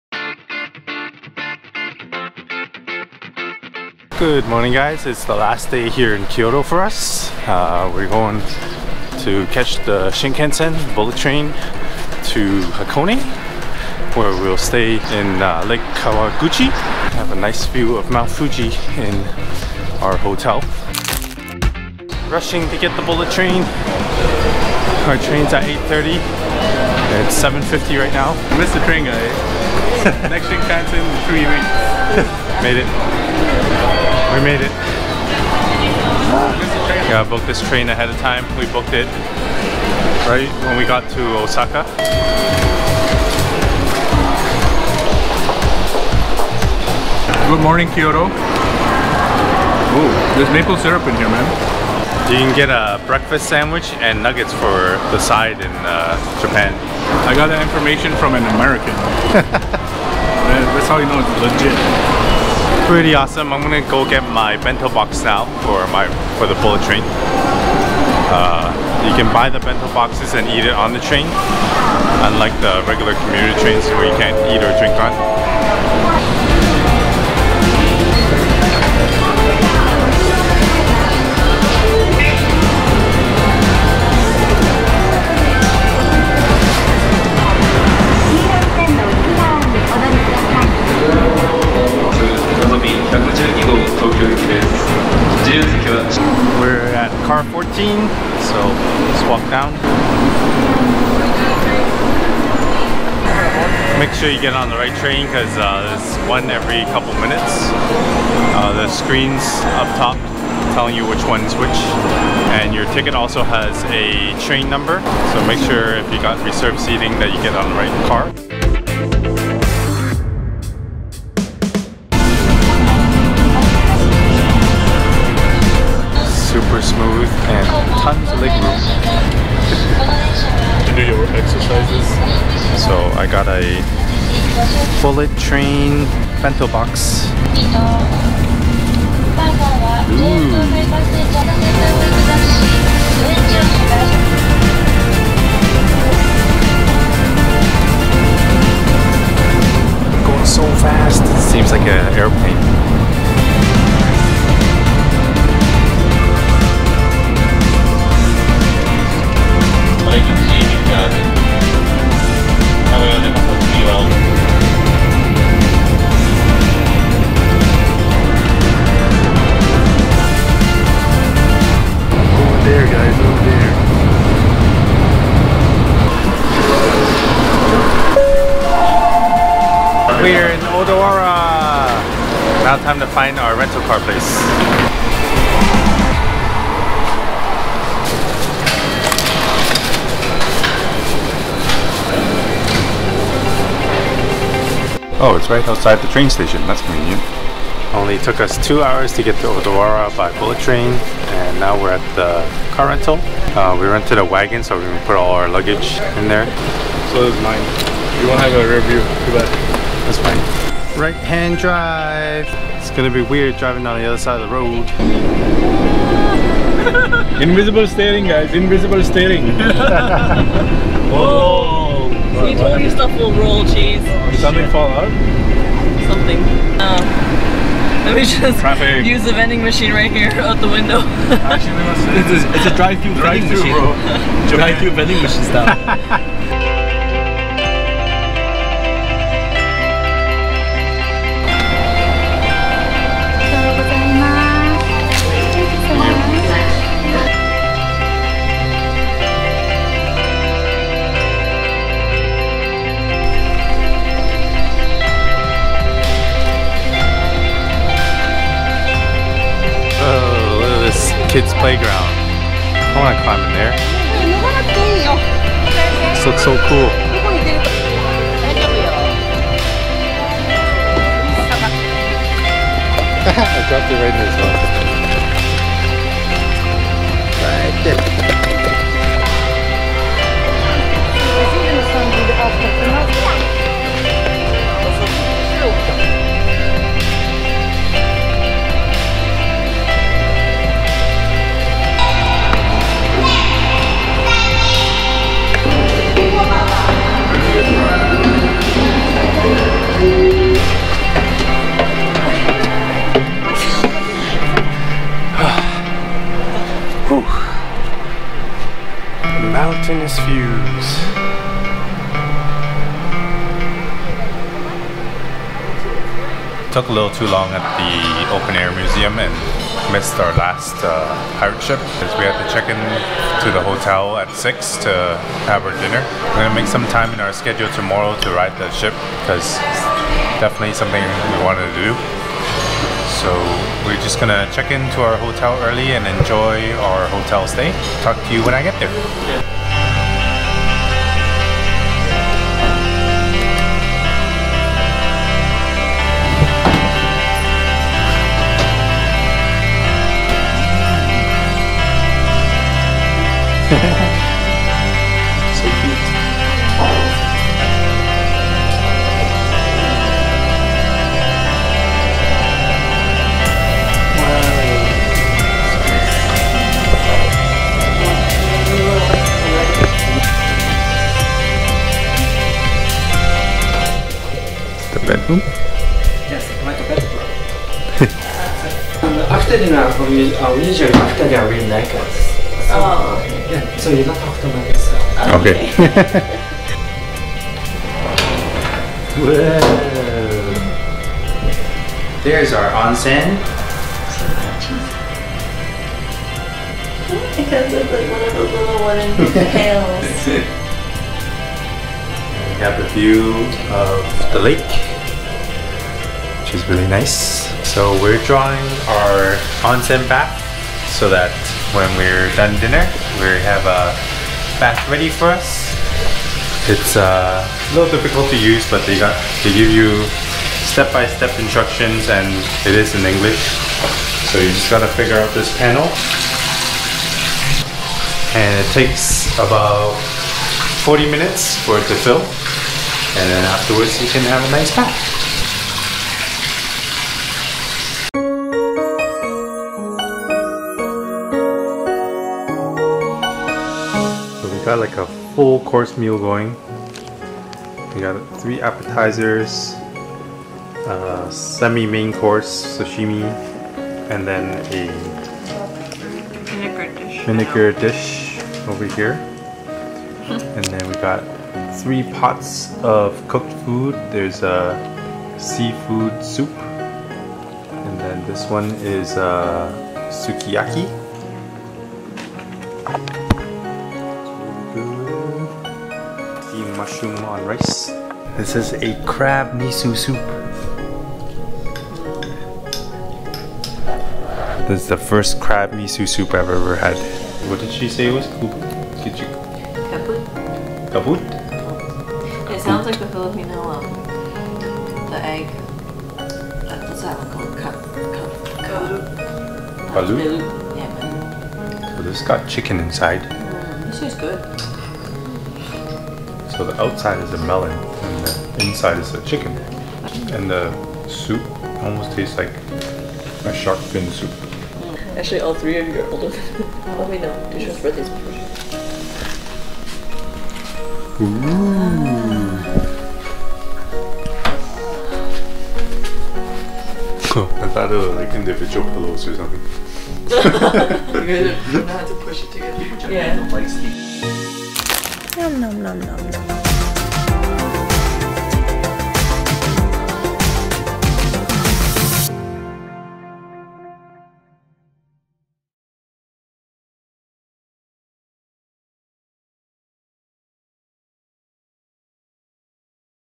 Hakone Open Air Museum, Shinkansen Bullet Train, Ryokan, Kaiseki dinner | Day 9 | Hakone, Japan
We left Kyoto and journeyed to Hakone using the Japan's super fast bullet trains, the Shinkansen.  Visited the Hakone Open Air Museum by Picasso and ventured to our hotel in the famous lake district of Lake Kawaguchiko.  It was our first time renting a car in Japan and driving on the right side but it wasn't too hard to get used to.  We splurged one night on our hotel here to have the best ryokan experience along with the traditional Kaiseki dinner.  This is said to be the most beautiful place to see Mt. Fuji but since we arrived at night, we'll have to wait till the morning to see if that's true.